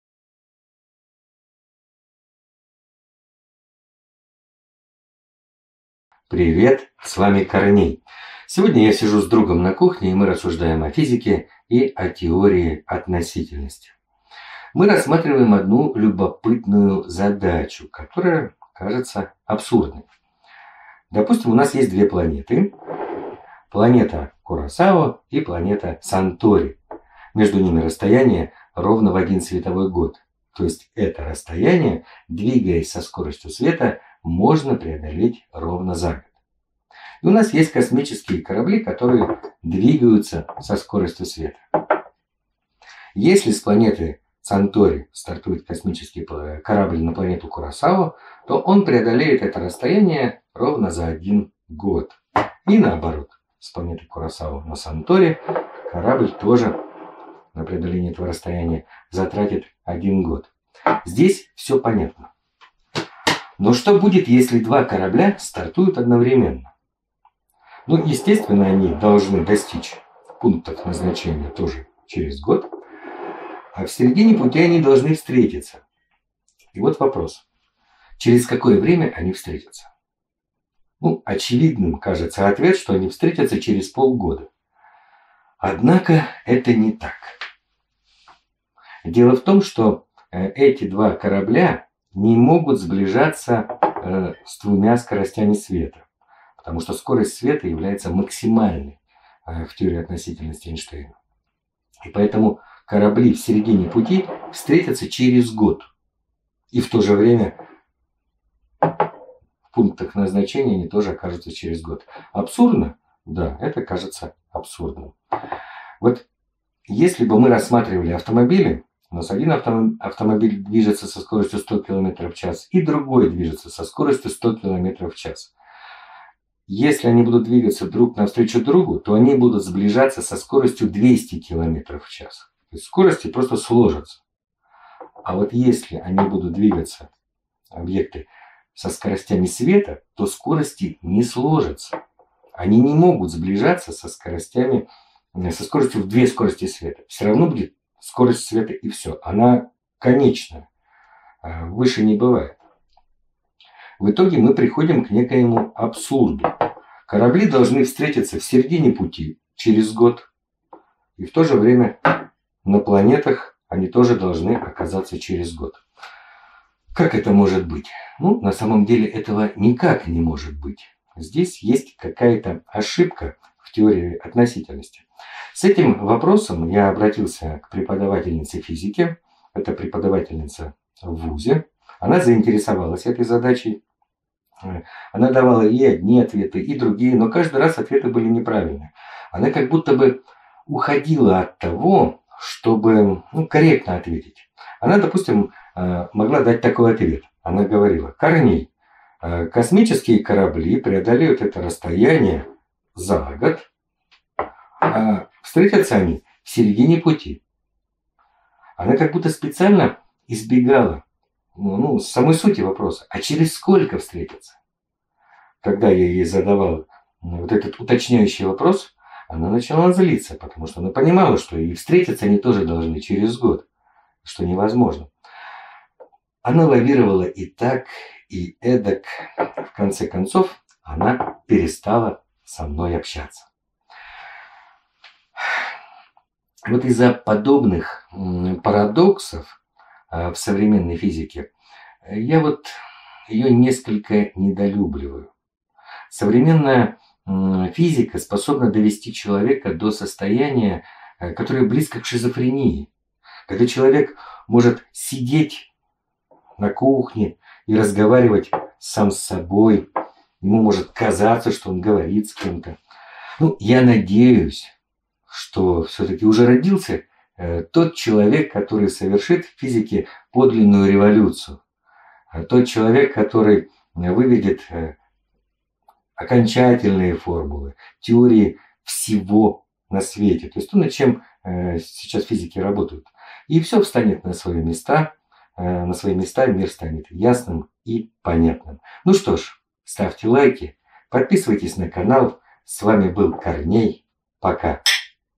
Как сделать теорию относительности (Theory of Relativity)
Progress is inevitable!
Прогресс неизбежен!

Современная физика полна противоречий. Теория относительности может привести к взаимоисключающим ответам. Когда физические опыты поставлены быть не могут, можно только предполагать какова будет реальность.